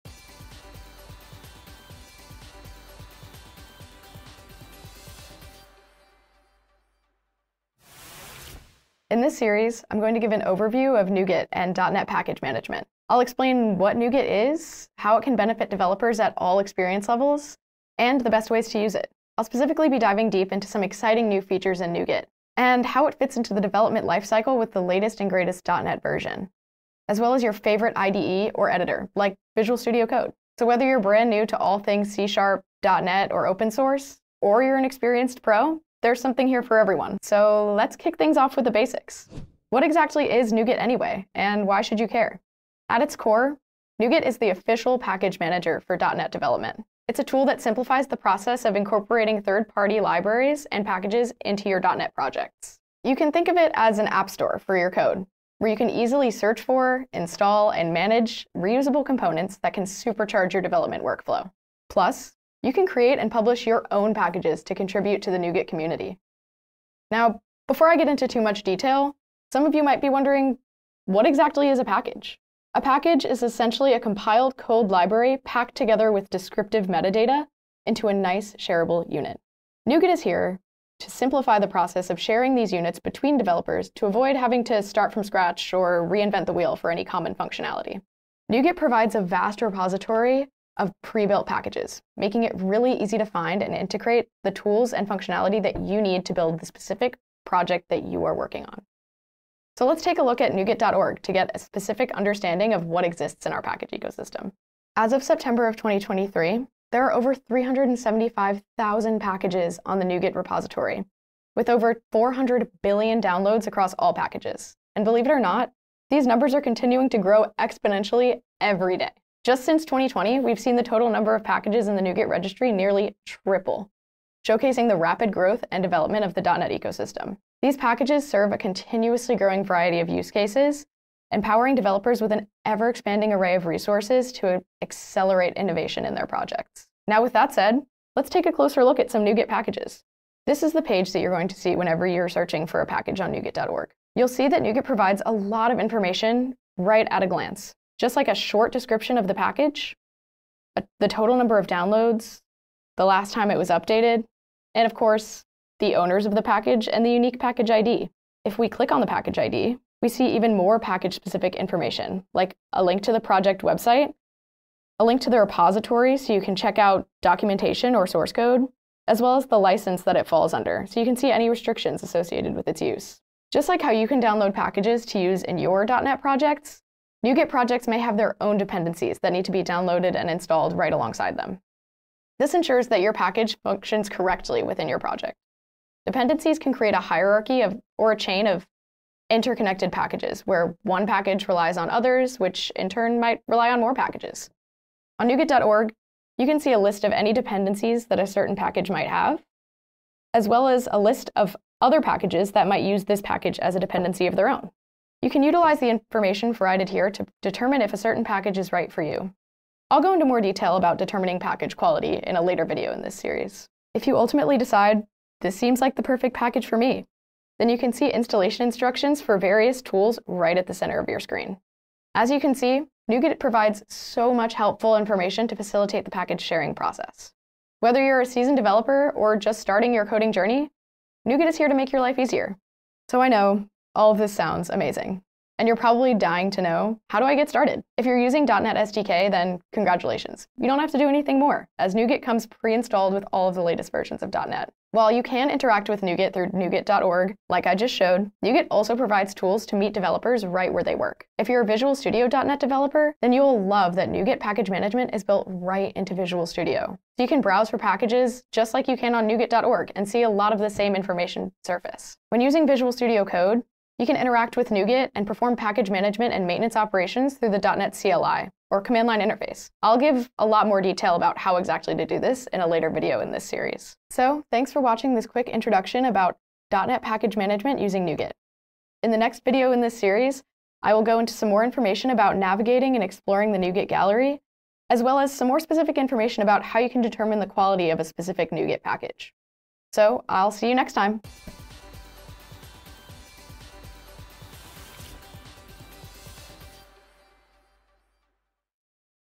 Introduction to .NET Package Management using NuGet [Pt 1] | NuGet for Beginners
Hi, friends! 👋🏻 Welcome to the .NET Package Management with NuGet for Beginners Series.

In this video series, Product Manager Allie Barry discusses open source package management using NuGet, the offical package manager tool for .NET. Allie demonstrates how to conduct package operations, such as installation and usage of a package in your project, as well as how to create, pack, and publish your own package to NuGet.org. Lastly, in this series, Allie discusses some best practices to follow when using NuGet as both a package consumer and a package author, as well as security features available in NuGet to help keep your data safe when using third-party packages.

In this introductory video, Allie gives a quick introduction to package management, as well as an overview of how NuGet works.

Links:
An Introduction to NuGet: What is NuGet and what does it do? | Microsoft Learn
Install NuGet Client tools: Installing NuGet client tools | Microsoft Learn